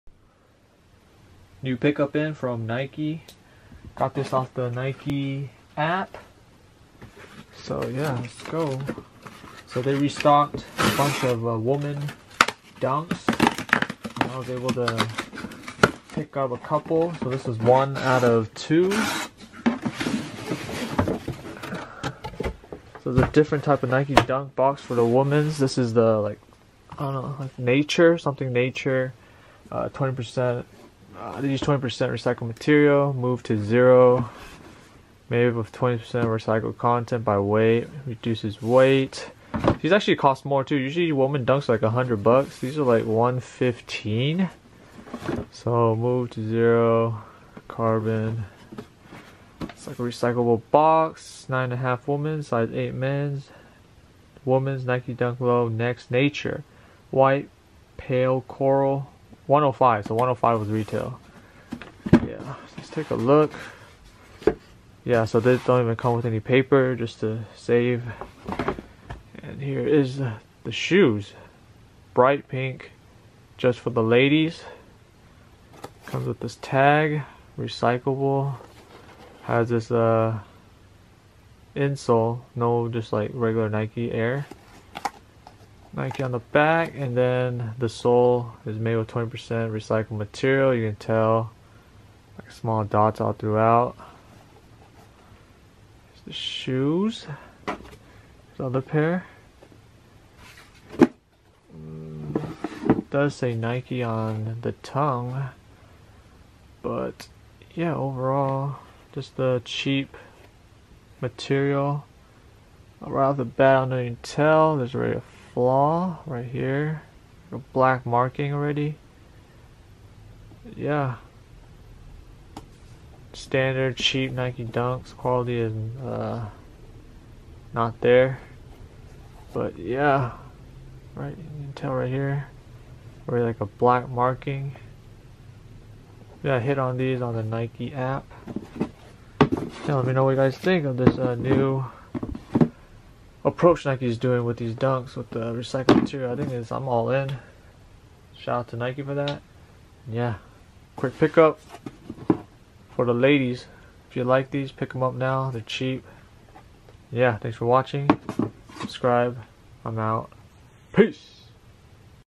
NIKE DUNK LOW NEXT NATURE PALE CORAL SNEAKER UNBOXING
Made with 20% recycled material. Hit on the nike app. Great women colorway.